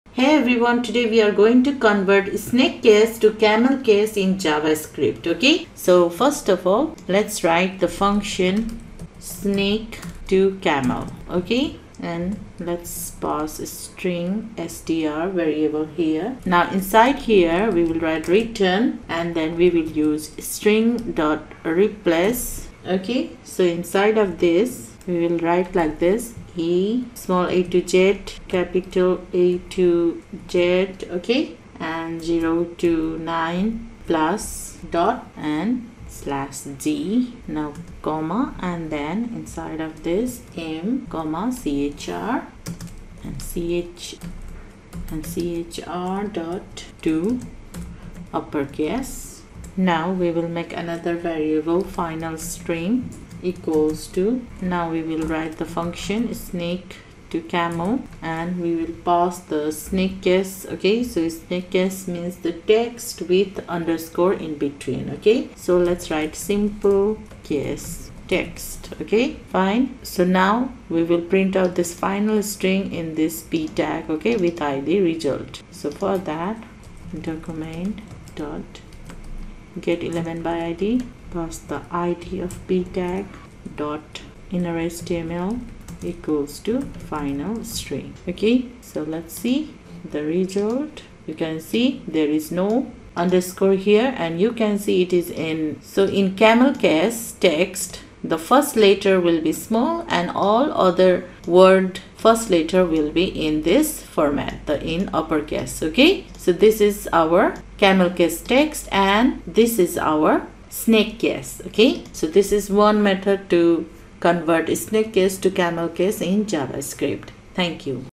Converting text from Snake Case to Camel Case with JavaScript
In this video, you will learn Converting text from Snake Case to Camel Case with JavaScript